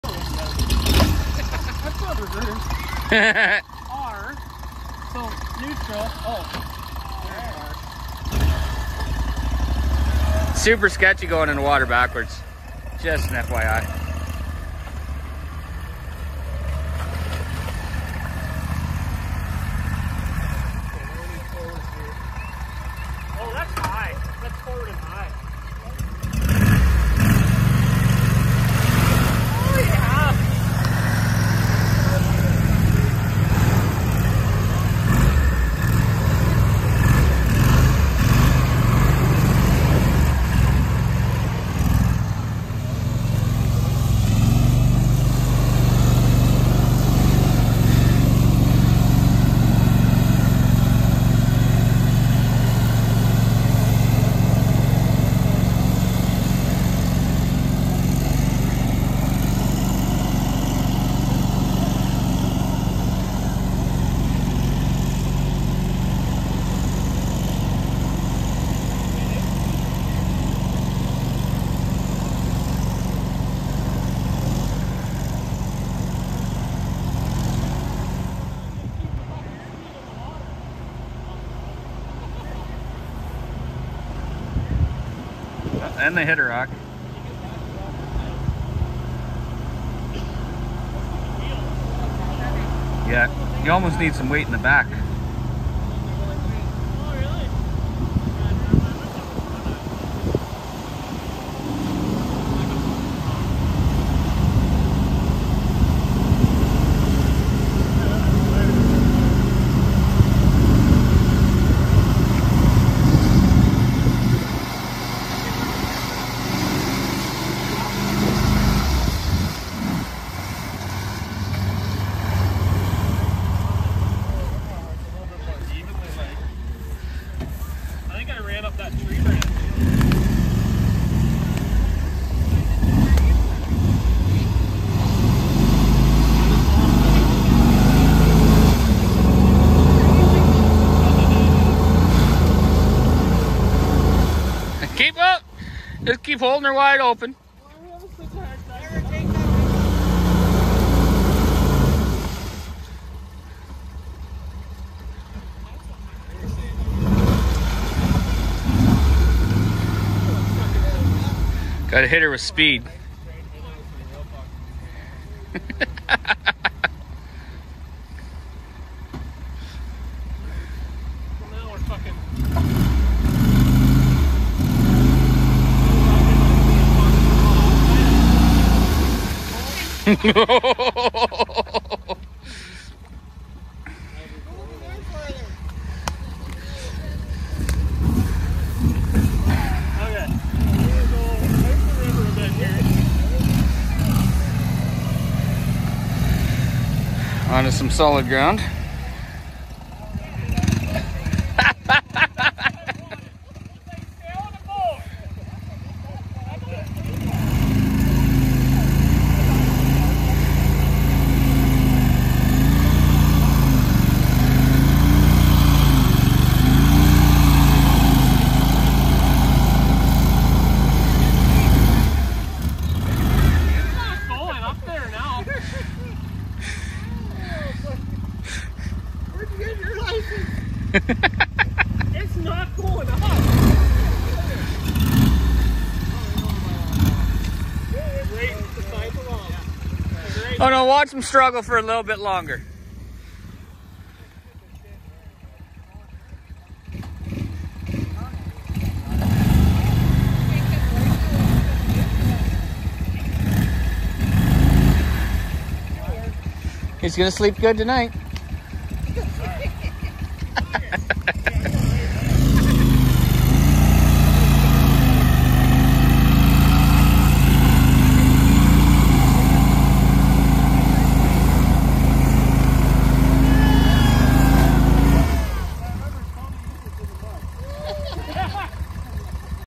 Argo 8x8 in the Water
Hello everyone, just a quick video of two adults in the 8x8 Argo floating about in the river. Thing struggles to get out of the water on muddy river banks.